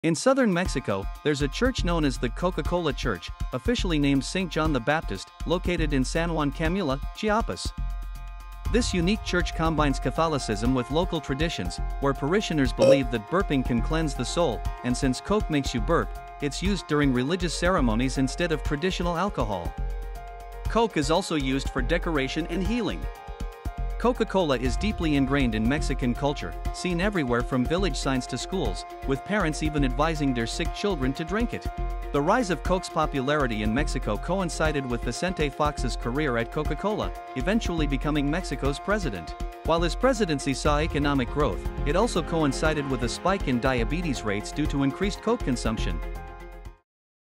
A Town Where Coca Cola Is Worshipped | Southern Mexico
Explore the unique and fascinating town where Coca-Cola isn't just a beverage—it's a way of life! where locals have turned their love for Coca-Cola into a cultural phenomenon.